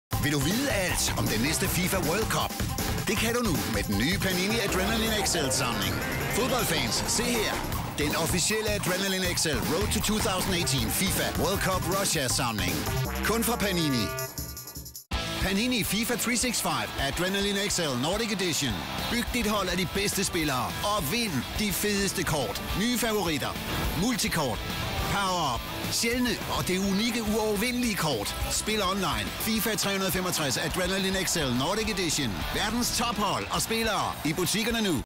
FIFA 365 Adrenalyn XL 2018 DK
FIFA 365 Adrenalyn XL-startpakke med FIFA 365 Adrenalyn XL-samlerkort, sæson 2018 Spark gang i din samling af FIFA 365-fodboldkortmed FIFA 365 Adrenalyn XL-startpakken for 2018-sæsonen! Startpakken fra FIFA 365 Adrenalyn XL indeholder et FIFA 365 Adrenalyn XL-samlealbum og andre fodboldlækkerier og er perfekt til enhver FIFA-fan, der gerne vil i gang med FIFA 365-samlerkort. FIFA 365 NordicEdition-boosterpakkerne er fyldt med fantastiske højdepunkter, bl.a. kort med nogle af verdens største hold og bedste fodboldspillere, 4 lokale hold, helt nye underserier og imponerende tegninger og design. Når du samler på FIFA 365-fodboldkort med FIFA 365-boosterpakkerne for 2018-sæsonen, kan du få fat på alle de populæreste spillere og hold og opbygge den mest imponerende samling af FIFA 365 Adrenalyn XL-fodboldkort nogensinde! Der findes 468 FIFA 365-samlerkort for 2018-sæsonen, så der venter tonsvis af fantastiske overraskelser på dig, bl.a. eksklusivt indhold, du kun finder i FIFA 365 Nordic Edition-samlerkortene. Få endnu mere sjov ved at samleog bytte med dine venner. Hvilke kort får du mon? Startpakken indeholder samlemappe, spillebræt, 5 boosterpakker (med 6 kort i hver), 2 Limited Edition-kort og 1 XXL Limited Edition-kort.